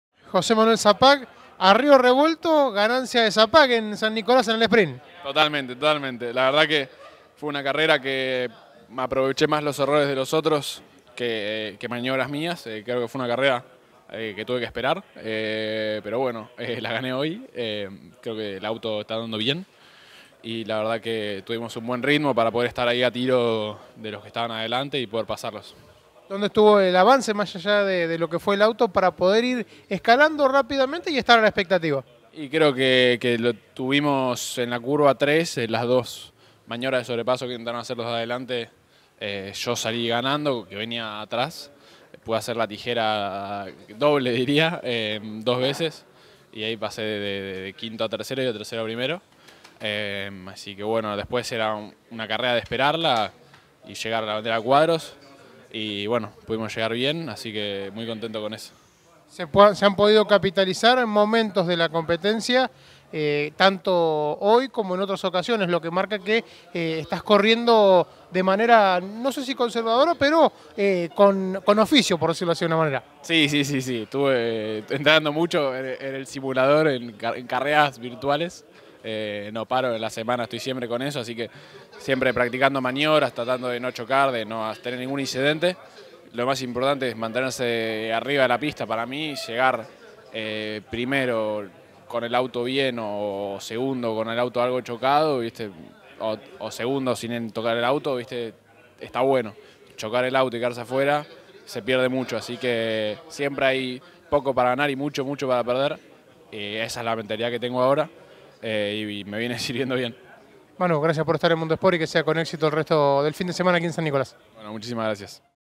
José Manuel Sapag - Sprint TC2000 San Nicolás 2019
Luego de haber ganado el Sprint del TC2000 en San Nicolás, José Manuel Sapag dejó sus conceptos en los micrófonos de Mundo Sport.